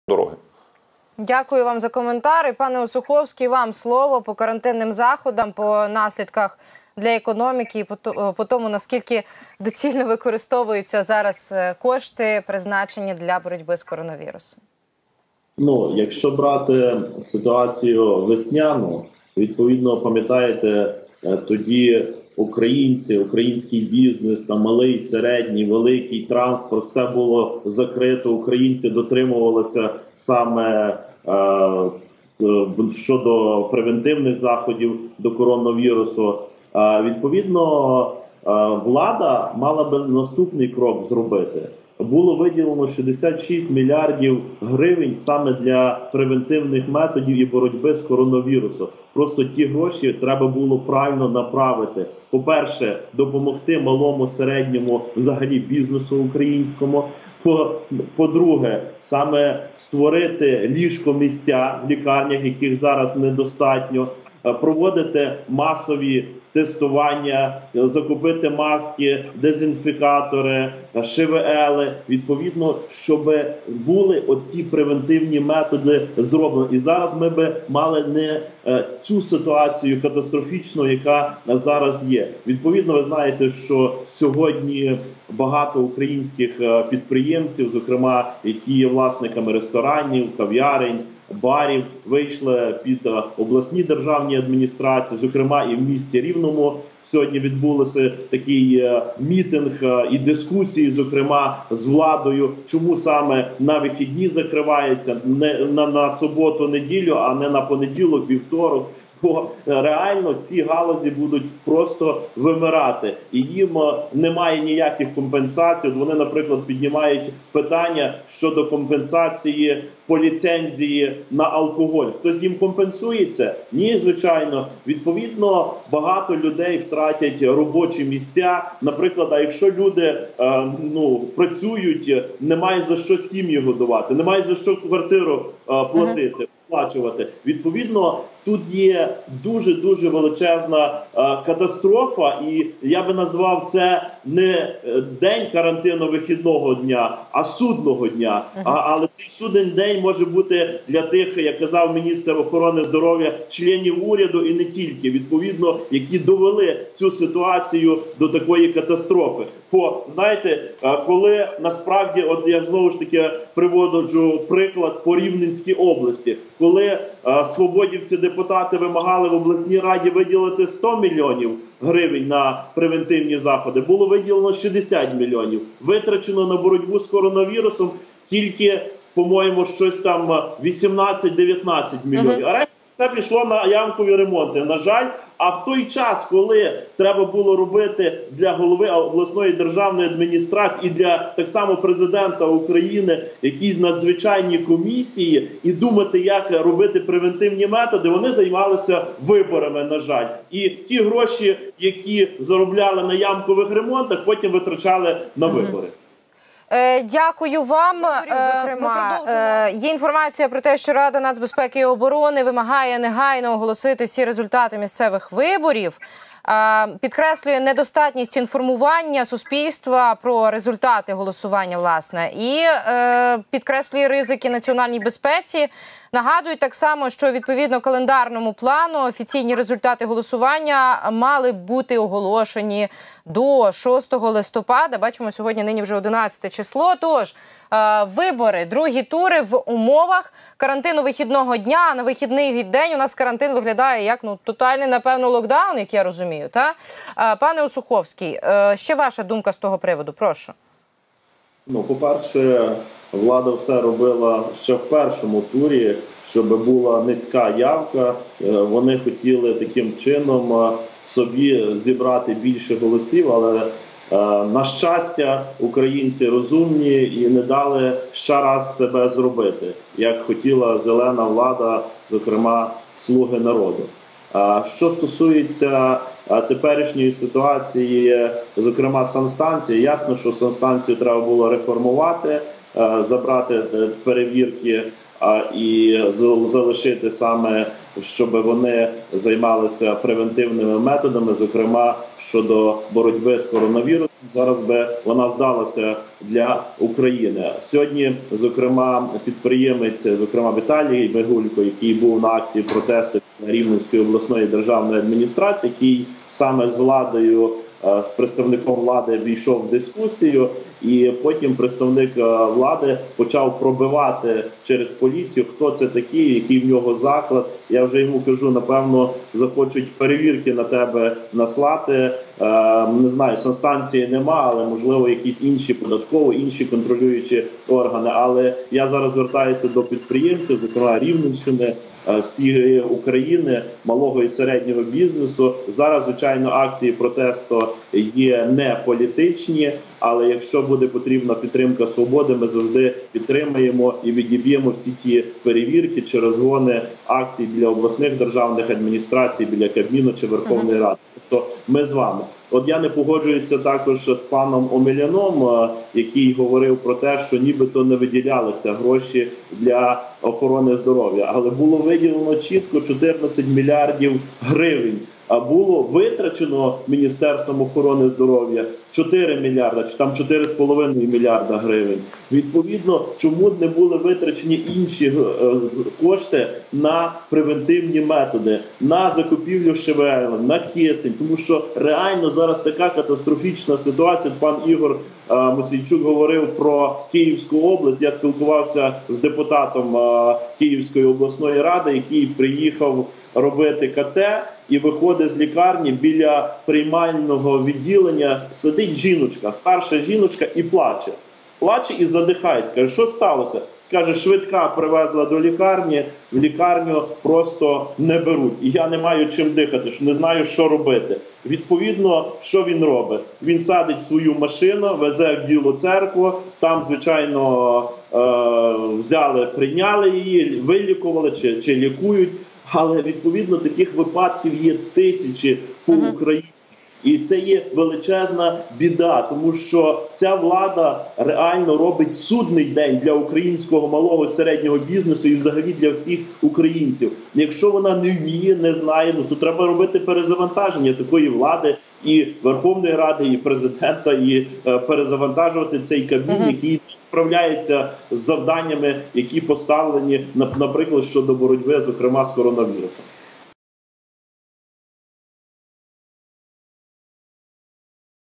Осуховський - Про введення карантину вихідного дня, про протести ФОПів, про критичний стан медицини
Про введення Урядом України карантину вихідного дня, про протести підприємців по всій країні, про критичний стан в медицині! Етер канал Еспресо 11.11.20р.